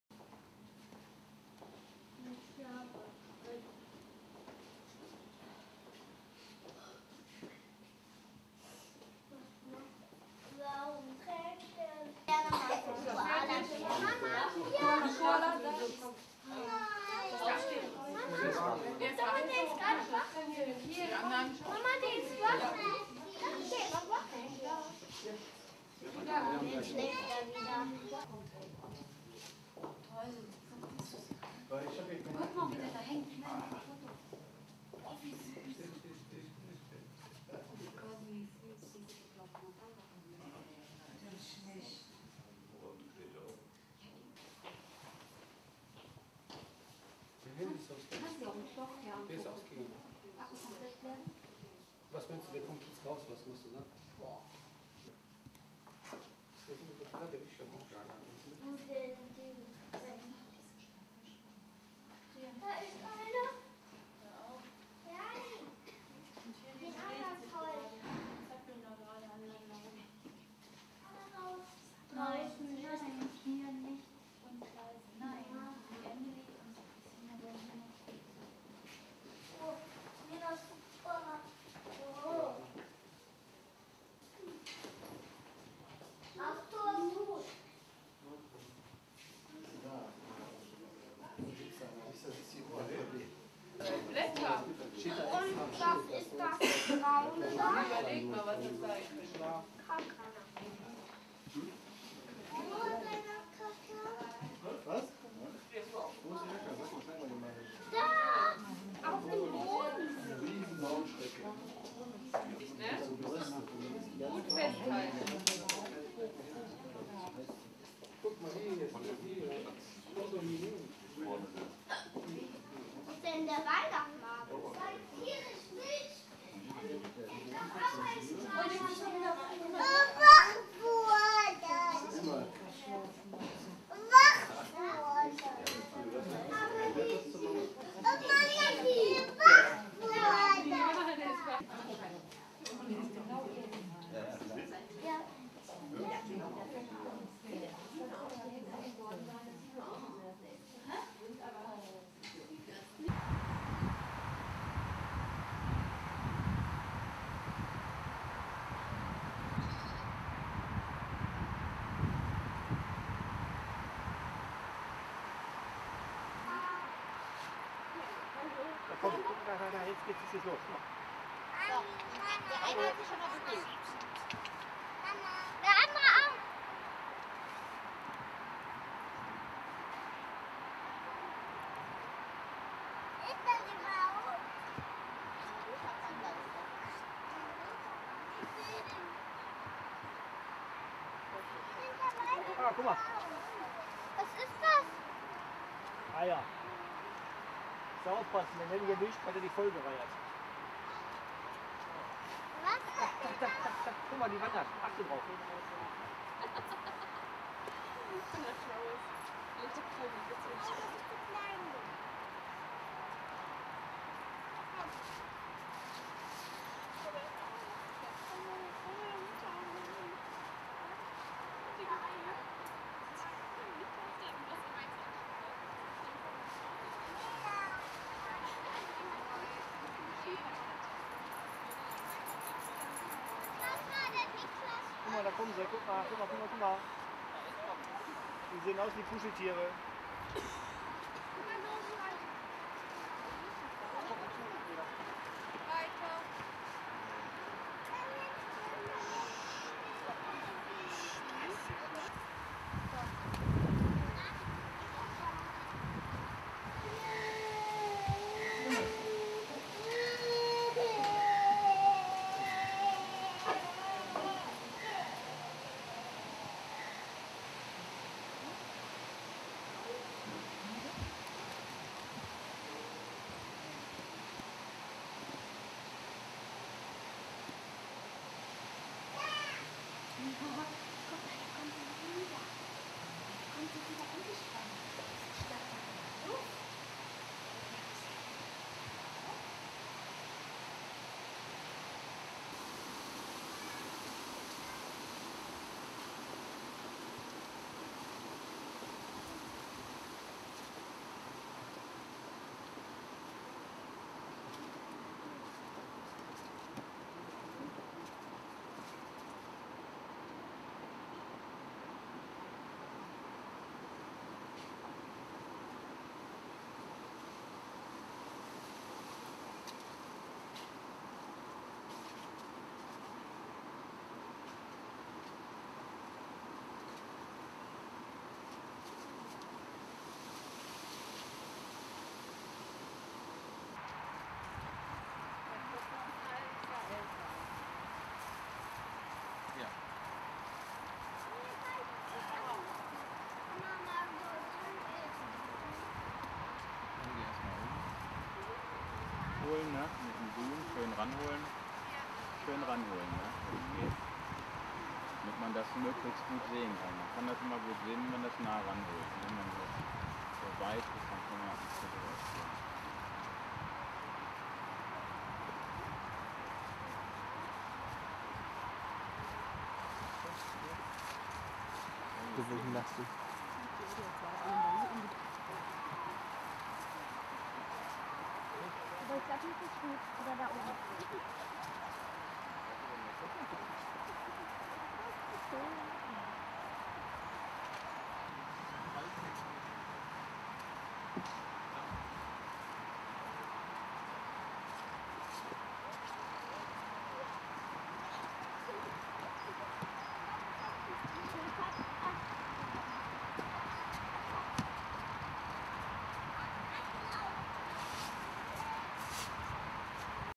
Plüsch im Zoo Duisburg
Beim letzten Besuch im Zoo Duisburg wurden einige der Tiere dezent mit der Kamera verfolgt: Koalas (und ein Koalaimitat), Rote Pandas, Erdmännchen, Äffchen und ein Amazonas-Delfin. Der ist der einzige ohne Plüsch, aber dafür sehr gelassen.